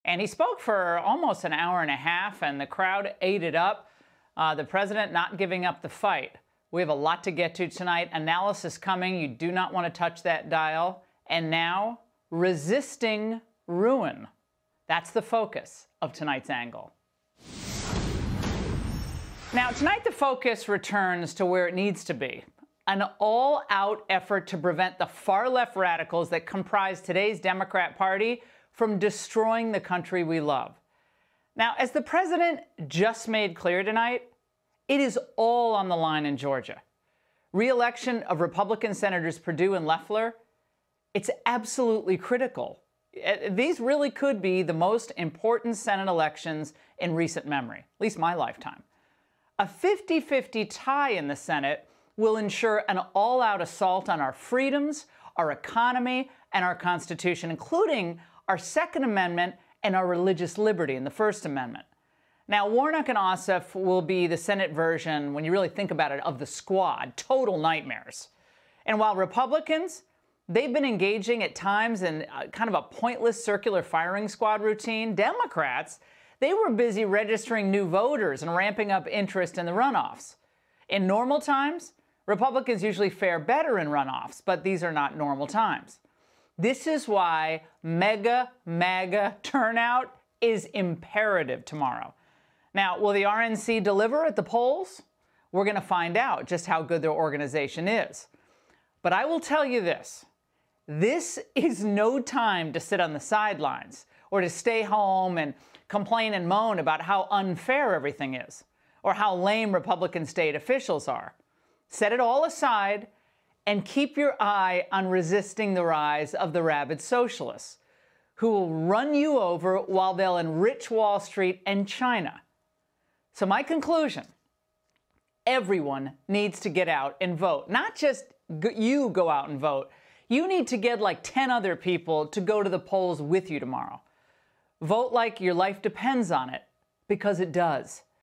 Ingraham: Everything is on the line
Laura Ingraham says the Georgia Senate runoff race may be the most important election in history.